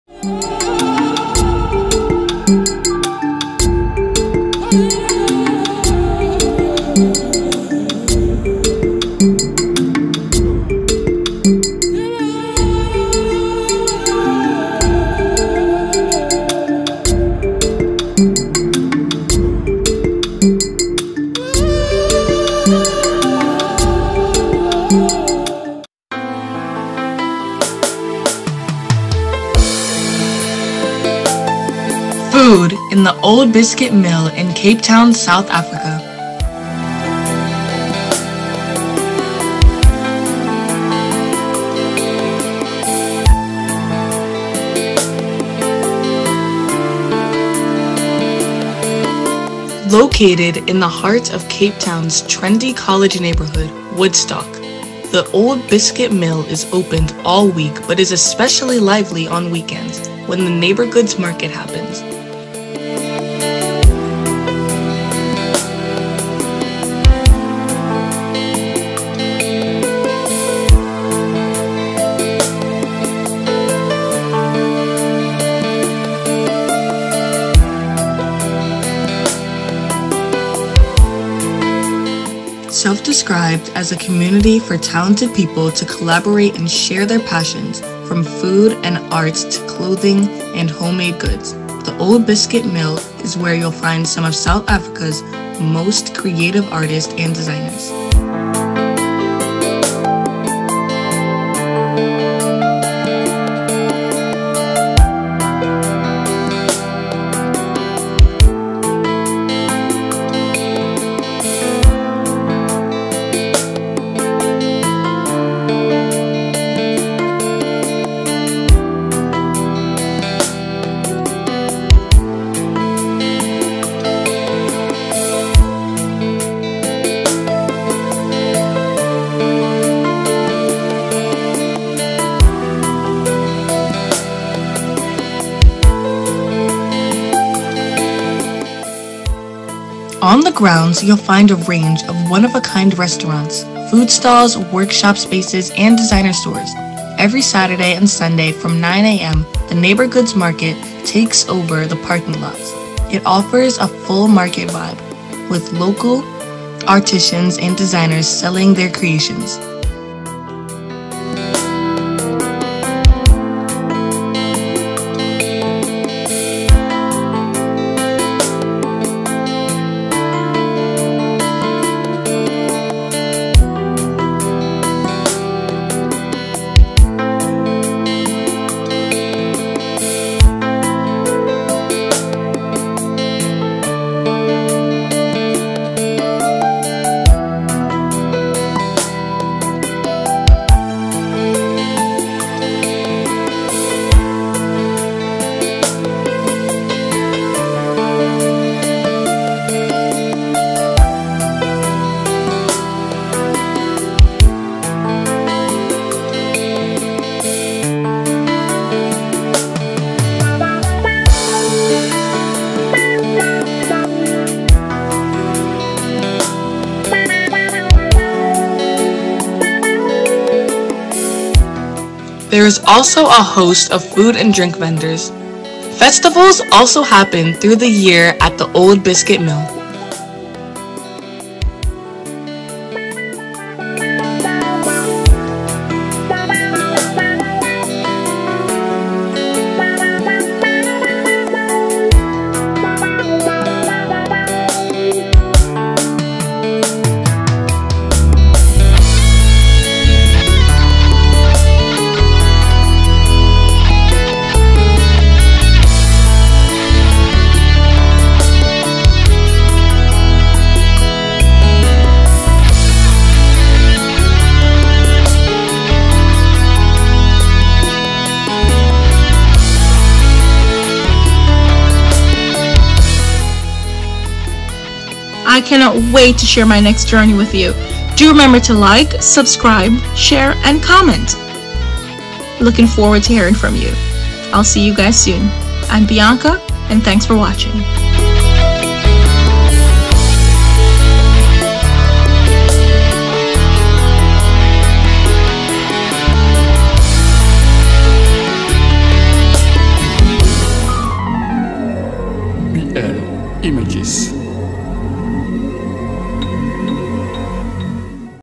Culinary Delights at The Old Biscuit Mill | Woodstock, Cape Town Food Tour
Join us as we explore the vibrant food scene at The Old Biscuit Mill in Woodstock, Cape Town! Known for its diverse selection of artisanal foods, gourmet delights, and fresh produce, this iconic location offers a sensory experience like no other. From savory snacks to sweet treats, local delicacies, and craft beverages, The Old Biscuit Mill is a must-visit destination for food lovers and travel enthusiasts alike. Whether you're a foodie or a curious traveler, this tour will leave you craving more.